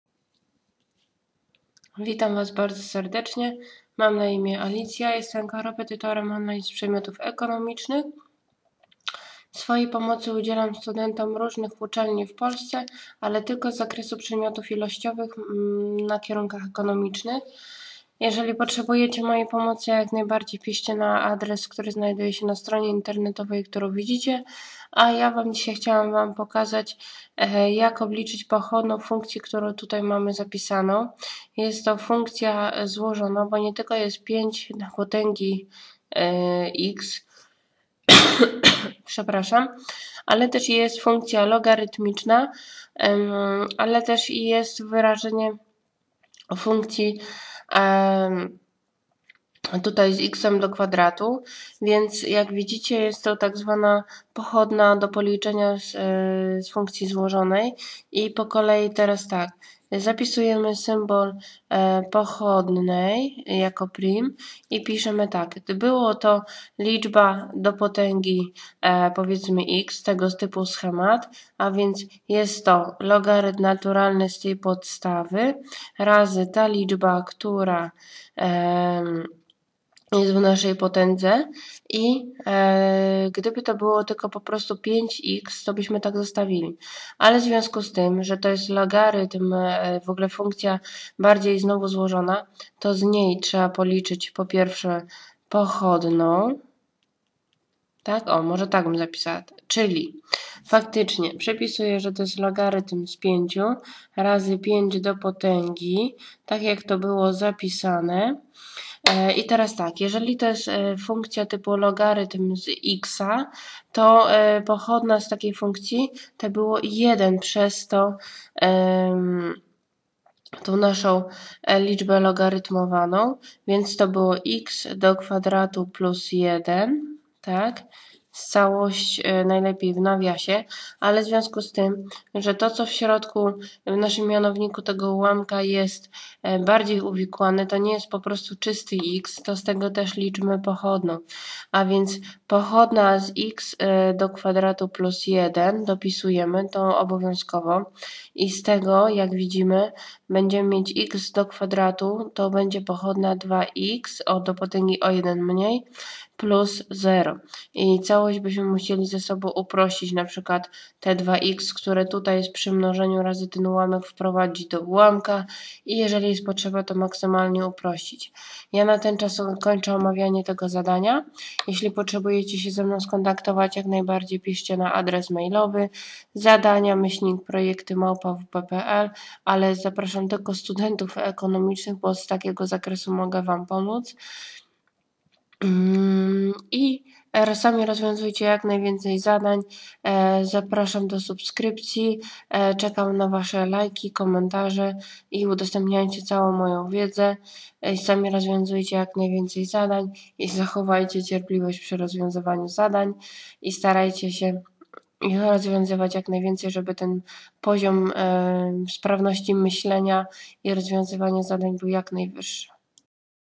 Analiza funkcji - pochodna funkcji złożonej ręcznie liczona - powtórzenie na studia - odc. 9
W filmie przypominam jak wykonać obliczenie pochodnej funkcji złożonej o 1 zmiennej, gdy funkcja składa się z logarytmu z funkcji liniowej.

Omawiam tak prosto, jak potrafię, aby było to zrozumiałe.

Zapoznaj się z innymi przykładami z playlisty "Powtórka".